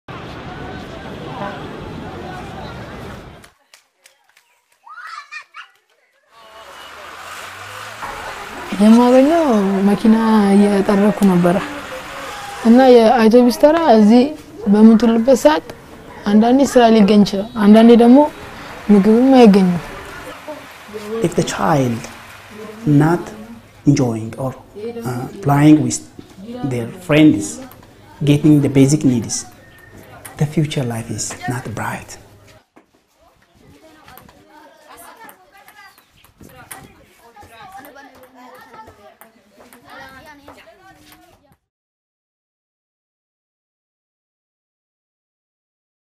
Tirusew aus Äthiopien / Save the Children
Kinder brauchen sichere Räume zum Spielen, um sich gesund entwickeln zu können. Mit der Kampagne „Let’s Play for Change“ fördert Save the Children gemeinsam mit der IKEA Foundation Begegnungszentren, in denen Kinder spielen und lernen können. Die Let's Play for a Change Kampagne startet in diesem Jahr zum 3. Mal.